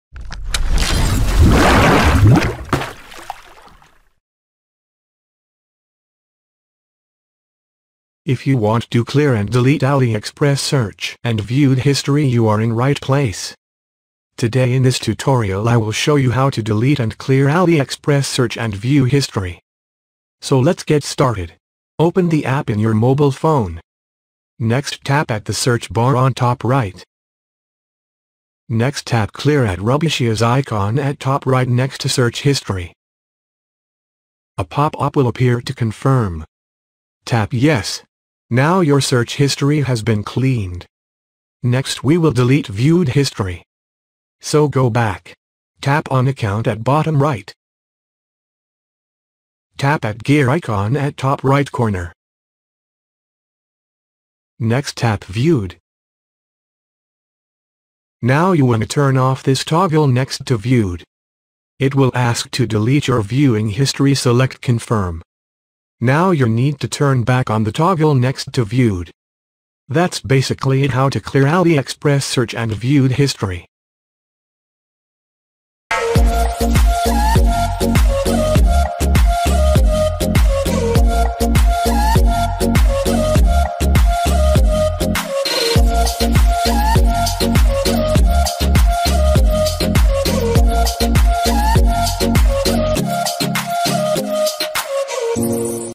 How To Delete Search And Viewed History On Aliexpress Mobile App
This tutorial works on androiid and iOS devices.

If you want do clear and delete aliexpress search and viewed history you are in right place. Today in this tutorial i will show you how to delete and clear aliexpress search and view history.